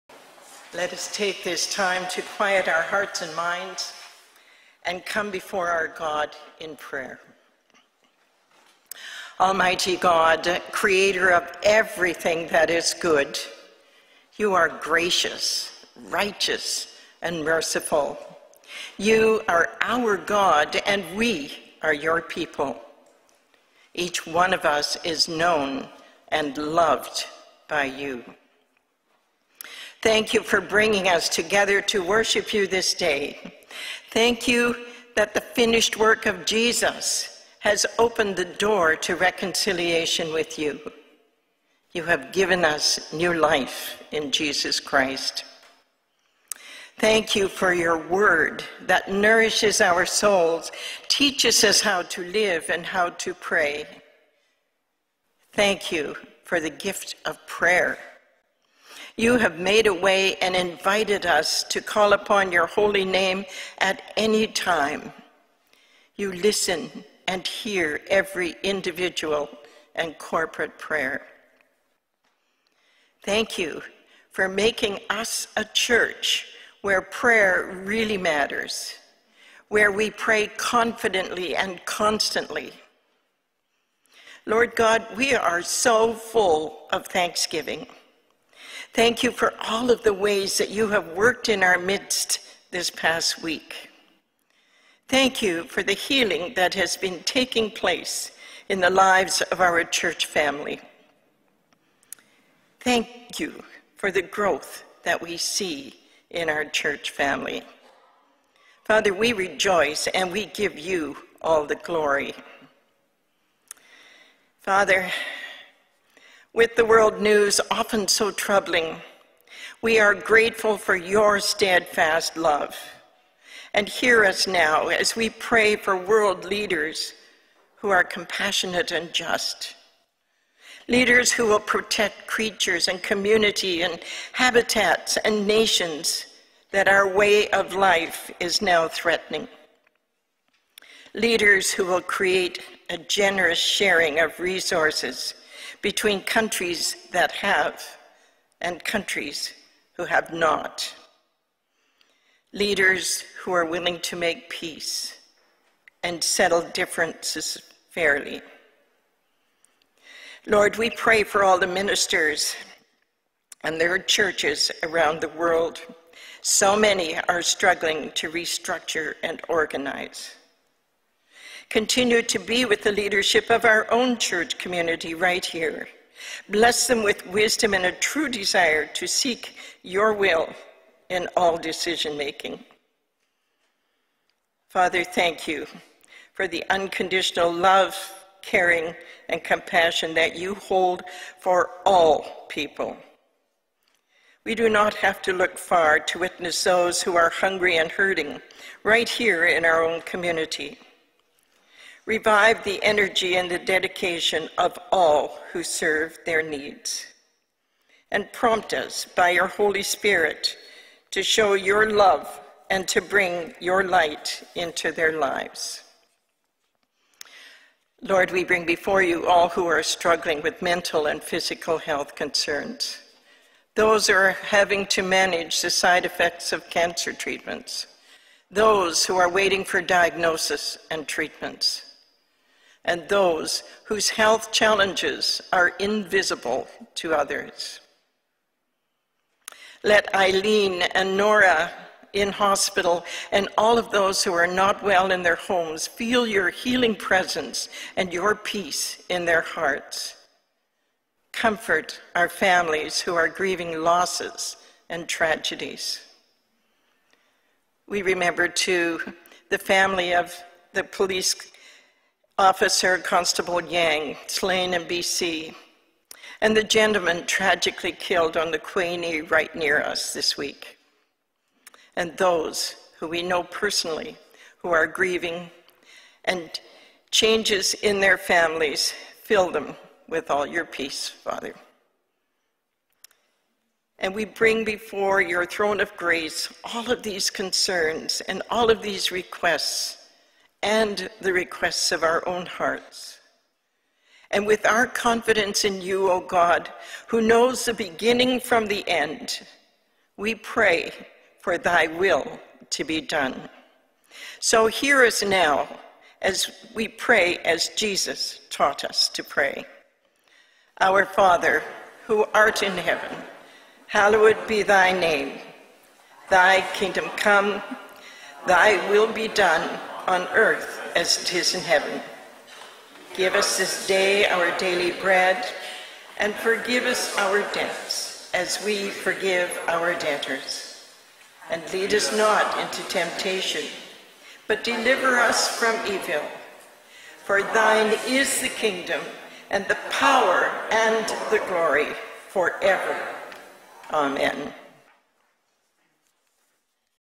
Pastoral Prayer | October 23, 2022
Listen to the Pastoral Prayer led by Chaplain Lynne Hieminga followed by the Lord's Prayer on this Sunday Worship Service @ Knox Presbyterian Church, Oakville.